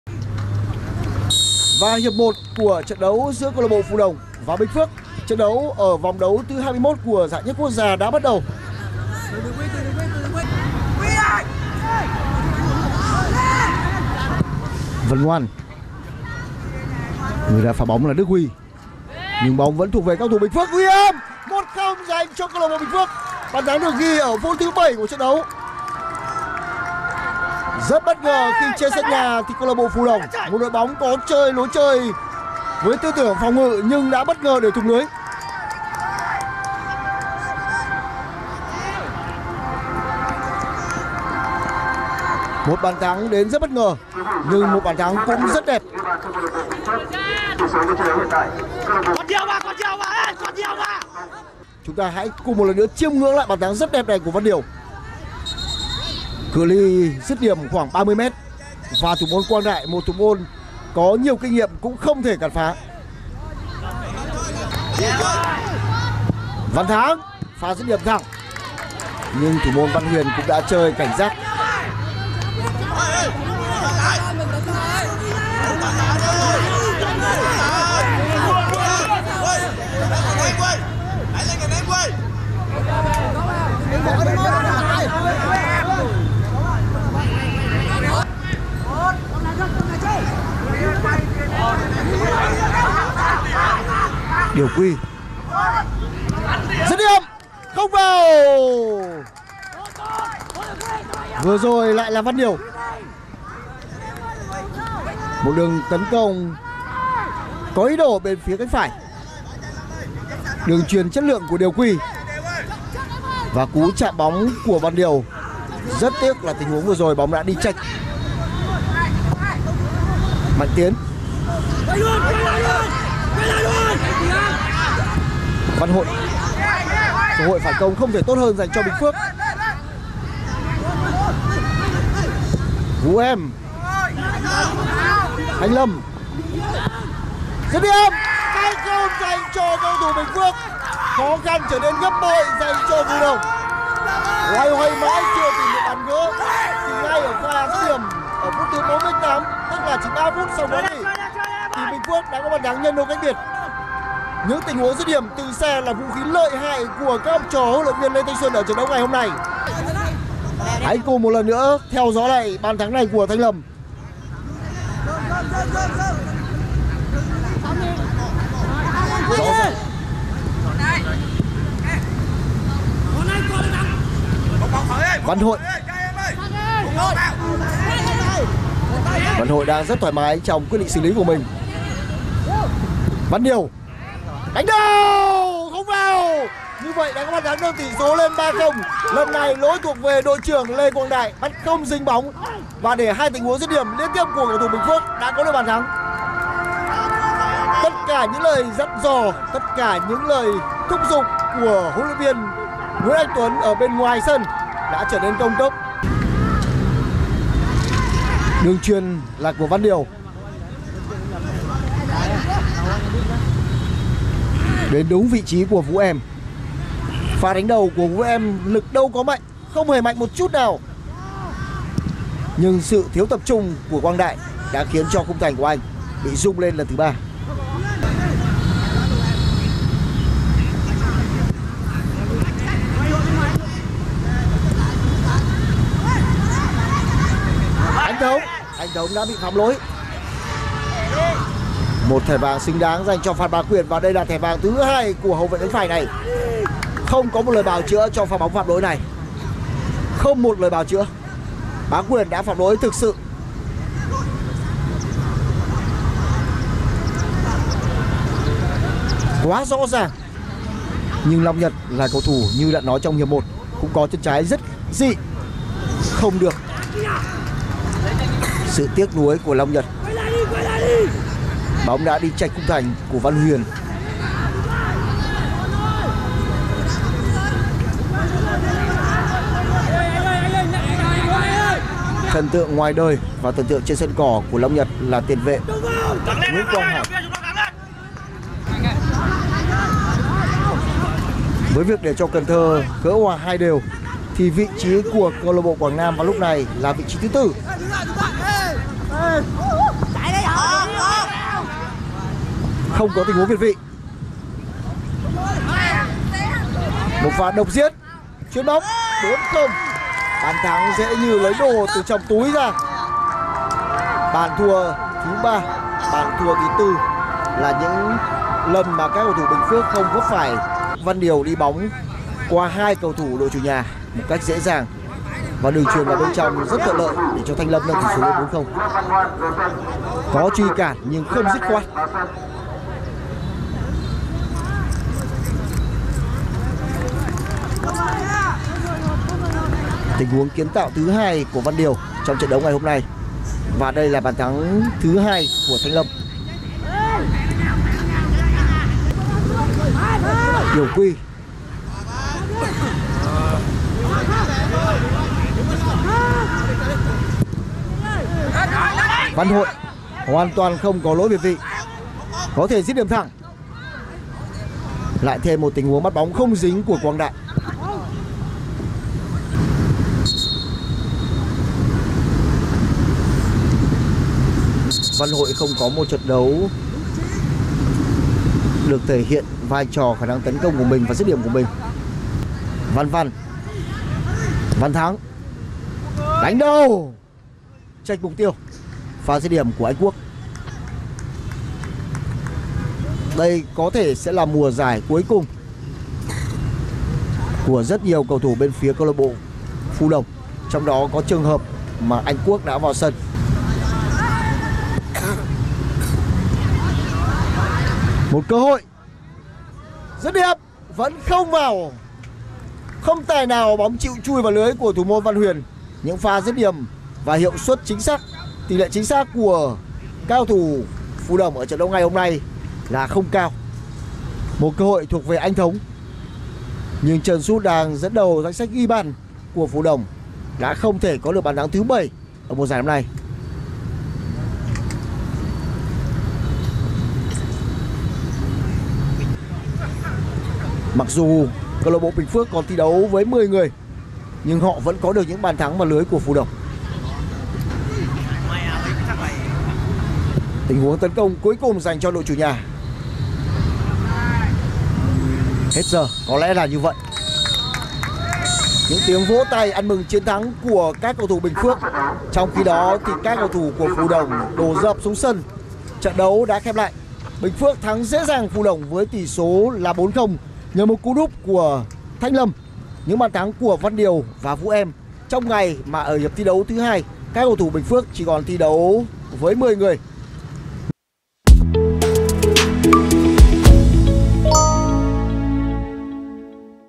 HIGHLIGHTS | PHÙ ĐỔNG – BÌNH PHƯỚC | Thi đấu nhạt nhòa, Phù Đổng duy trì chuỗi trận không thắng
NEXT SPORTS | HIGHLIGHTS | PHÙ ĐỔNG – BÌNH PHƯỚC | Thi đấu nhạt nhòa, Phù Đổng duy trì chuỗi trận không thắng